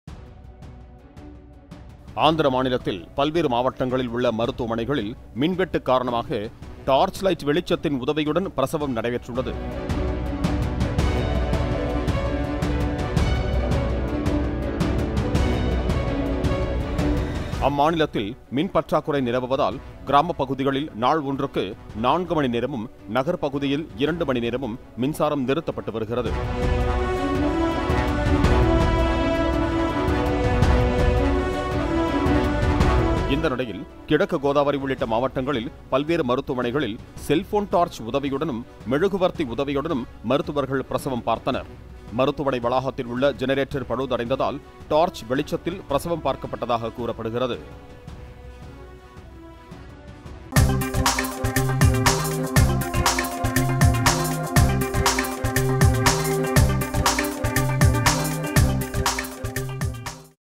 மருத்துவமனைகளில் மின்வெட்டு.. டார்ச் உதவியுடனும், மெழுகுவர்த்தி உதவியுடனும் பிரசவம் பார்த்த பரிதபம்!
ஆந்திர மாநிலத்தில் பல்வேறு மாவட்டங்களில் மின்வெட்டு

மருத்துவமனைகளில் டார்ச் லைட் வெளிச்சத்தின் உதவியுடன் பிரசவம்

மின் பற்றாக்குறையால் மாநிலத்தில் பல்வேறு மாவட்டங்களில் மின்வெட்டு